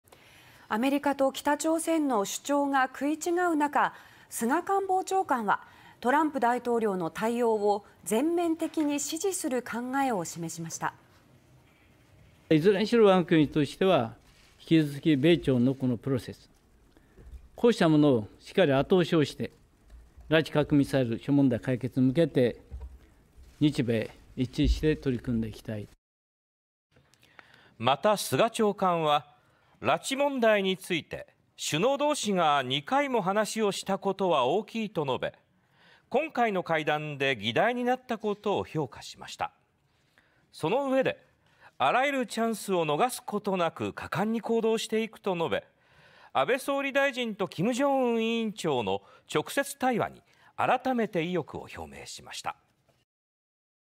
「米朝のプロセス後押し」 拉致問題言及を政府評価(19/03/01)
アメリカと北朝鮮の主張が食い違うなか、菅官房長官はトランプ大統領の対応を全面的に支持する考えを示しました。　菅官房長官：「いずれにしろ、我が国としては引き続き米朝のプロセスを後押しして拉致、核・ミサイル諸問題の解決に向けて日米一致して取り組んでいきたい」　また、菅長官は拉致問題について「首脳同士が2回も話をしたことは大きい」と述べ、今回の会談で議題になったことを評価しました。そのうえで、「あらゆるチャンスを逃すことなく果敢に行動していく」と述べ、安倍総理大臣と金正恩委員長の直接対話に改めて意欲を表明しました。